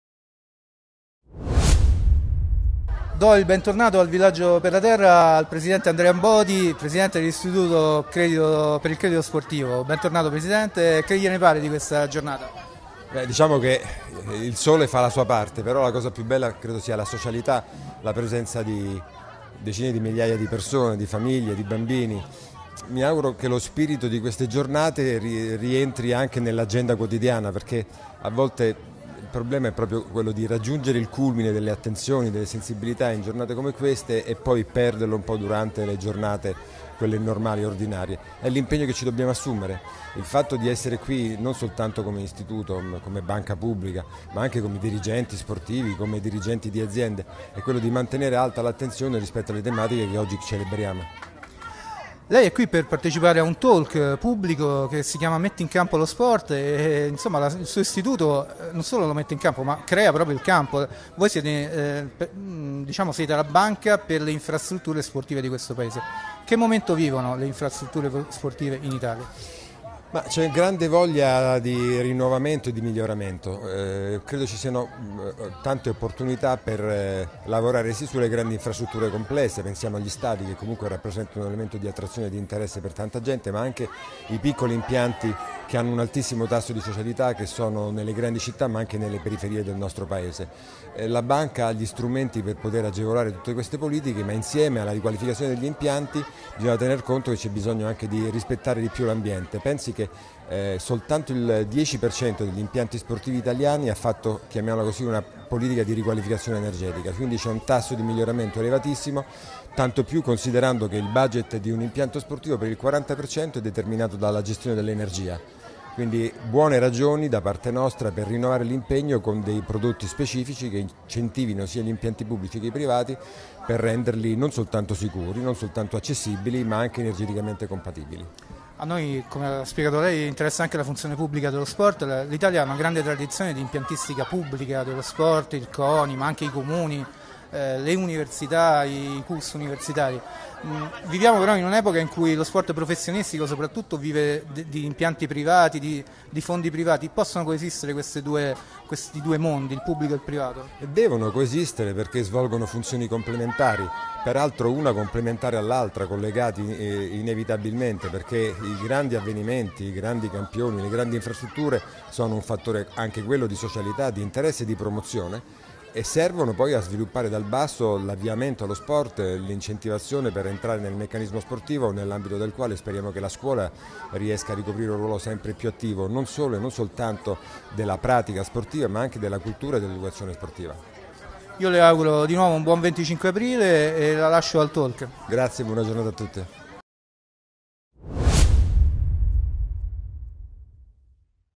Abodi (ICS): "Per gli impianti sportivi pubblico e privato devono coesistere"
Andrea Abodi, presidente dell'Istituto per il Credito Sportivo, intervistato al Villaggio per la Terra 2019 a margine del talk "PEACE - Metti in campo lo Sport!" (25 aprile - Galoppatoio di Villa Borghese).